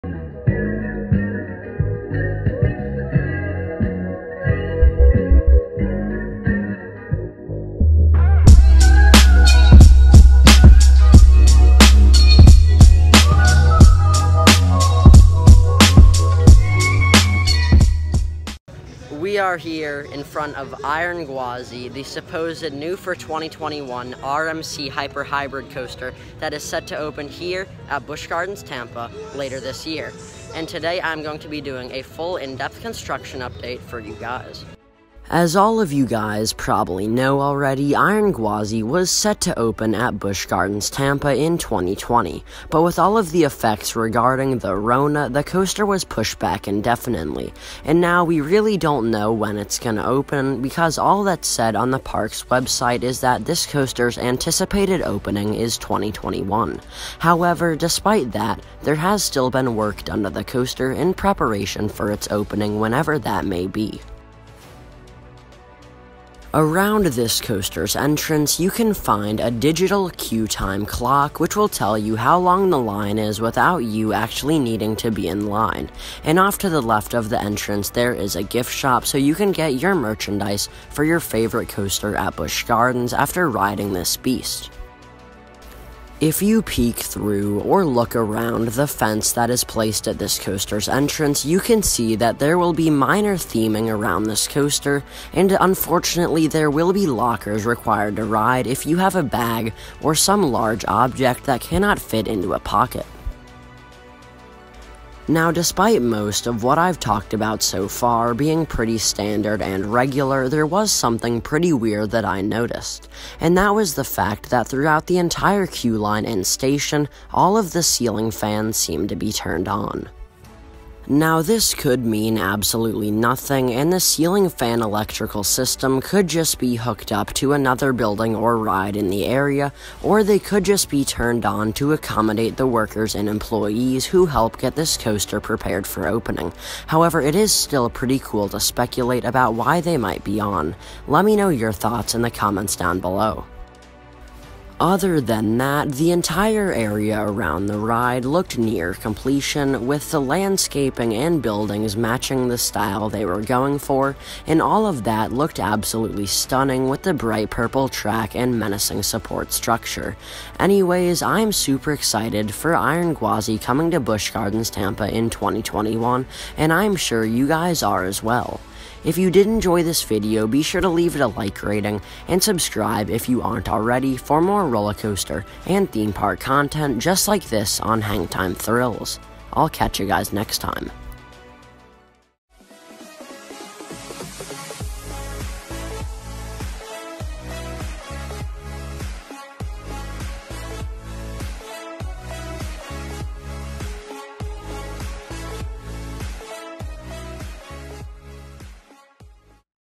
Iron Gwazi CONSTRUCTION UPDATE April 2021 - Busch Gardens Tampa 2021 Hybrid Coaster
Originally set to open in 2020, this new for 2021 RMC Hyper Hybrid Coaster looks to be absolutely incredible. Iron Gwazi will be one of the worlds greatest coasters no doubt, and here is an in-depth construction update of the coaster!

My Gear:

iPhone 7
iPhone 11
iPhone 8+
iVue Rincon 1080P